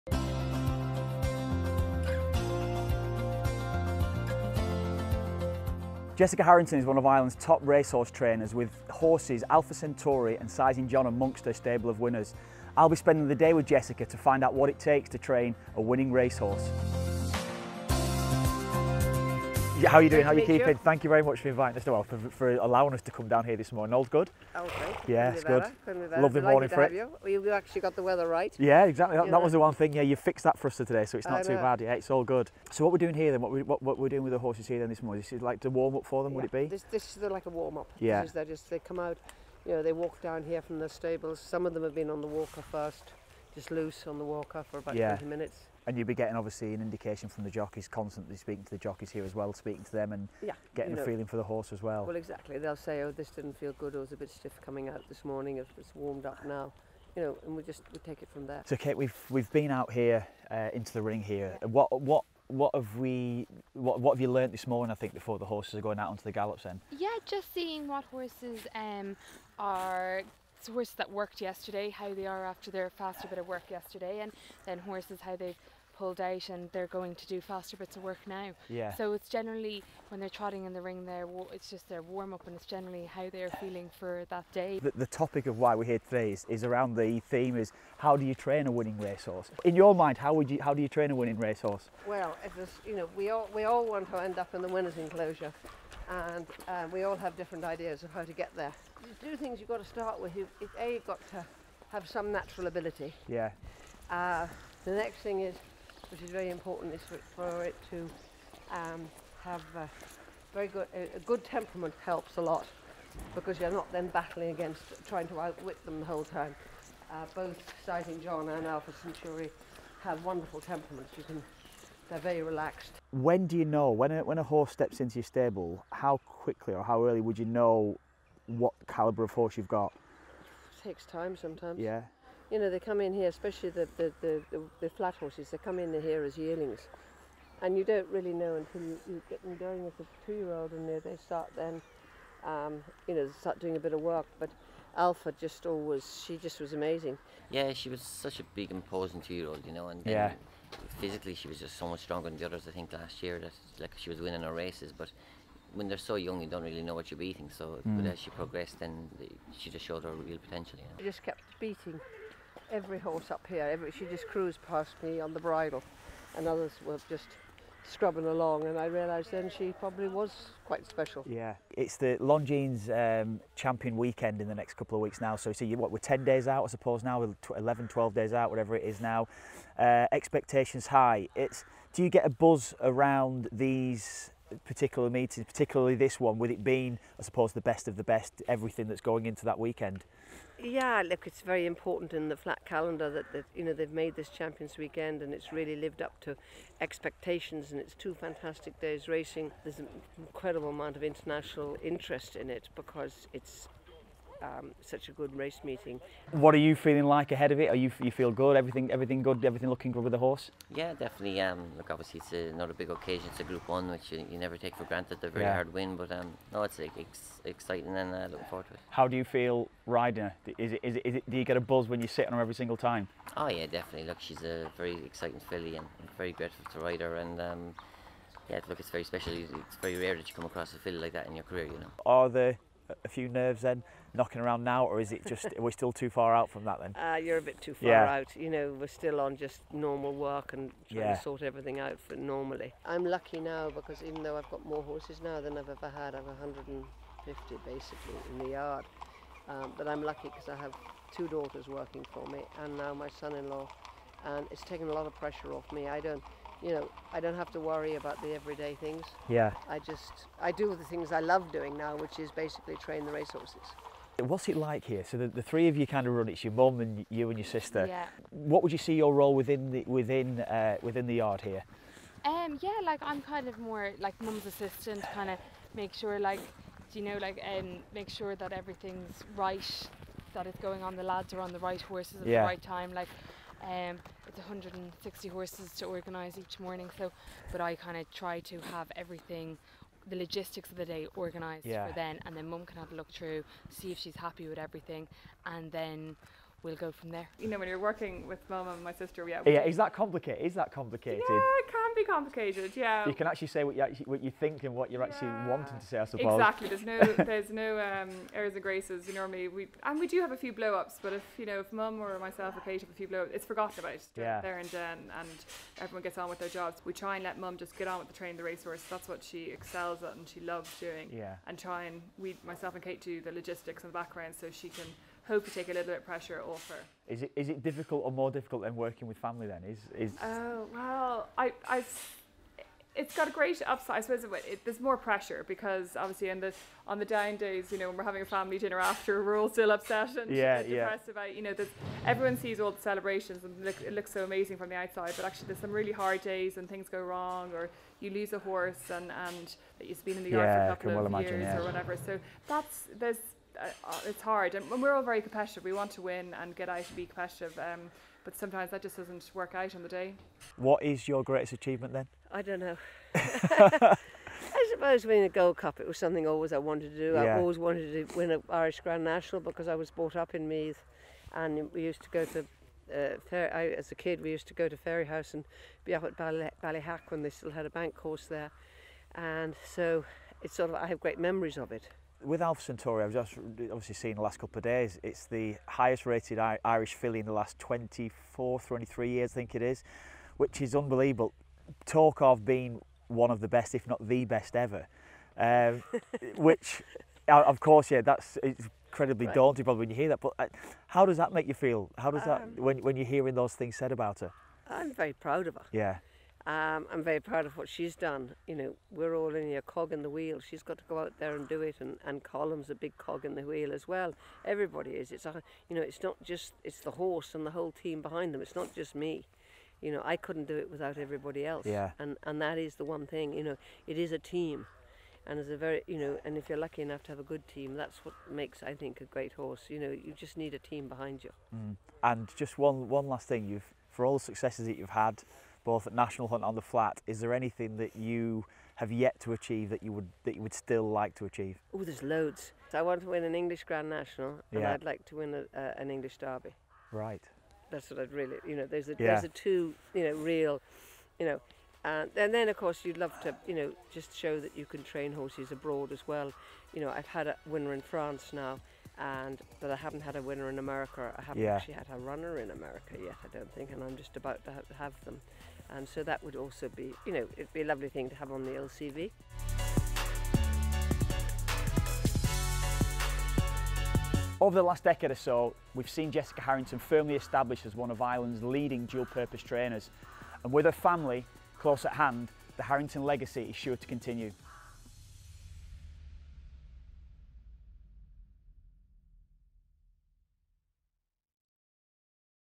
Kevin Kilbane discovers how to train a race-winning horse
Former Ireland international footballer Kevin Kilbane meets renowned race-horse trainer Jessica Harrington to get an insight into what it takes to train a winner

Our racing coverage is brought to you in association with Horse Racing Ireland